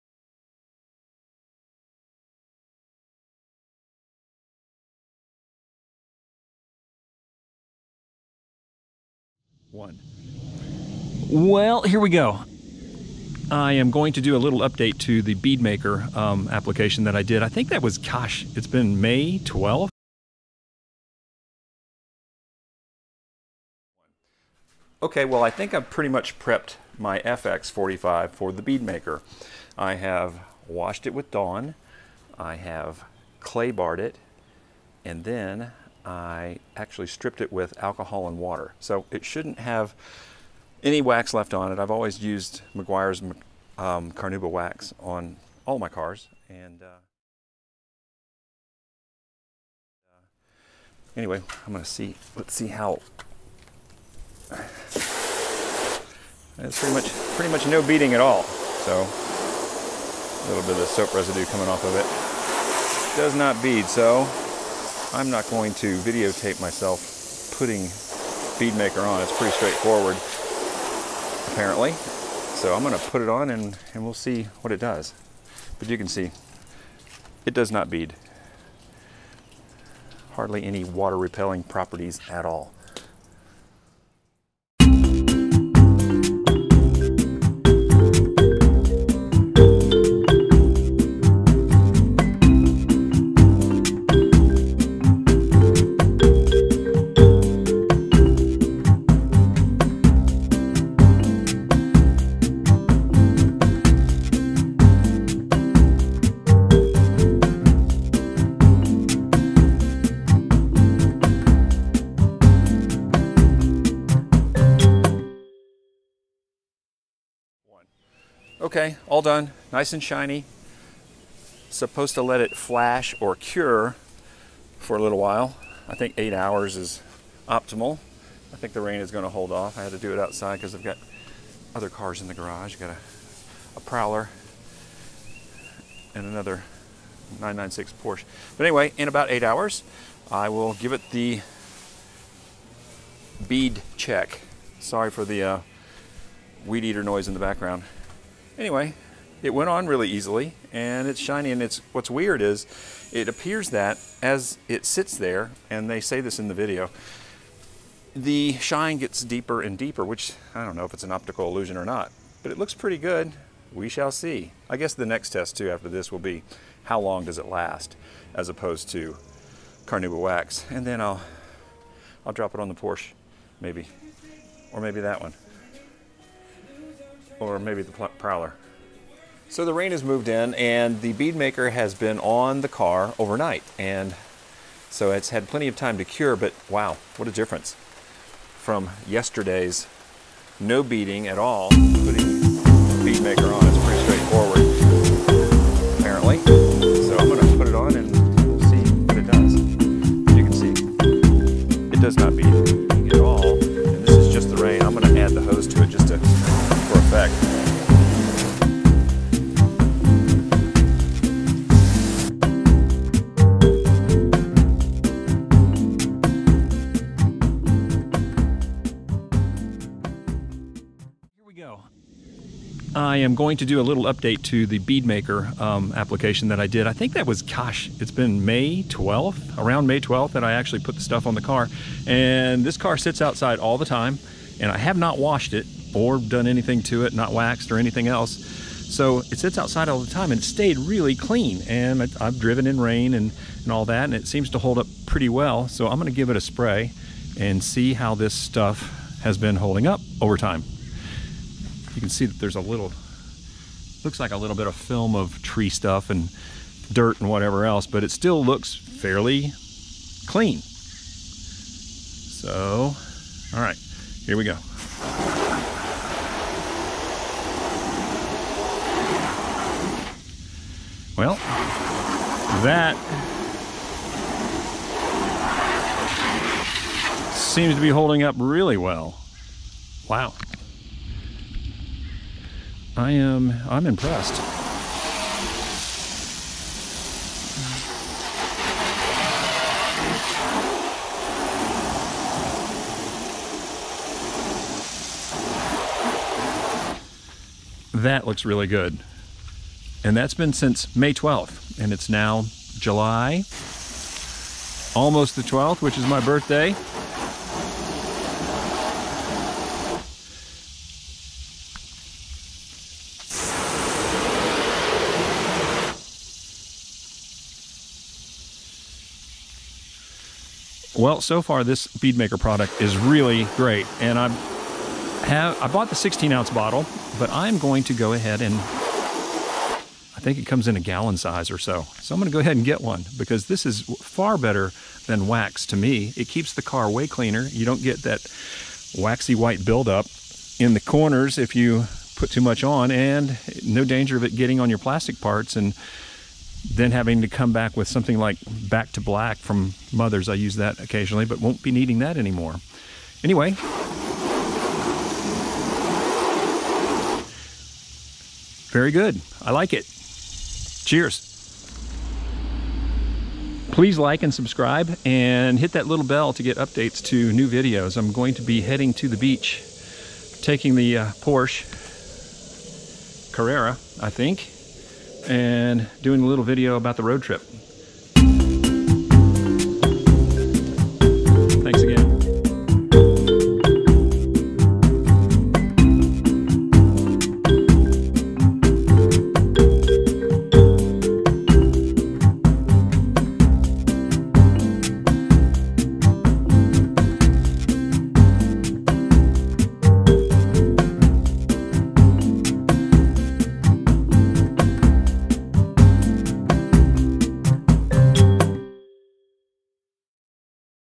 Bead Maker UPDATE! IS IT DURABLE??
This video is about Bead Maker and whether it is a durable product. so far?